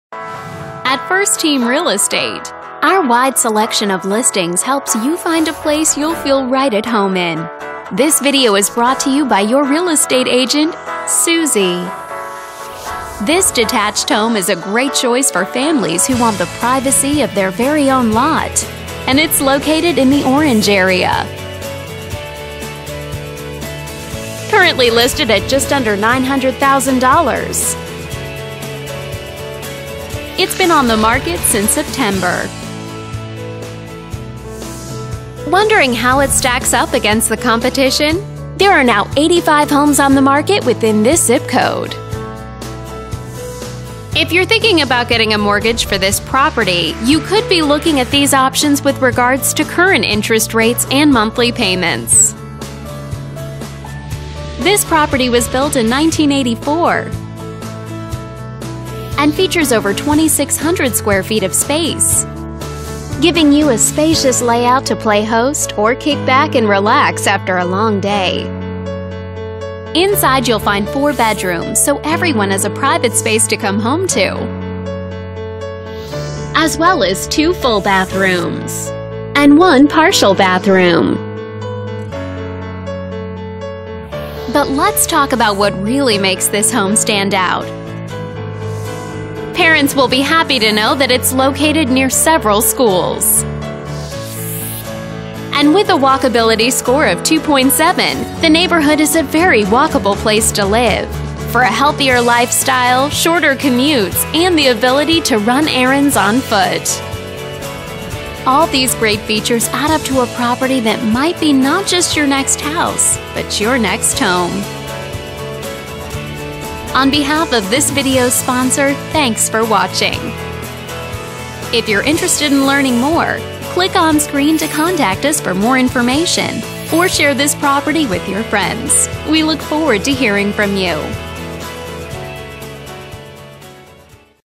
2018 North Lake Mead Circle
Beautiful Autumn Ridge executive home located on a quiet culdesac in Mabury Ranch. This is a wonderful property that shows just beautifully!  The kitchen was remodeled a few years ago and includes lovely cabinetry, built-in refrigerator, corian counters and quality appliances.  The two upstairs bathrooms were more recently remodeled with quality cabinetry, granite and fixtures. The entry to the home is elegant with soaring ceilings and lots of natural light.  There are also French Doors in the Family Room and Living Room.   The oversized lot/yard is picturesque with a built-in BBQ and Counter, Koi Pond, and multiple sitting areas.  There is a huge side yard that could easily accommodate a pool, sport court, etc.  The front yard is huge and gorgeous!  This is great family home and truly a unique property. The photos say it all!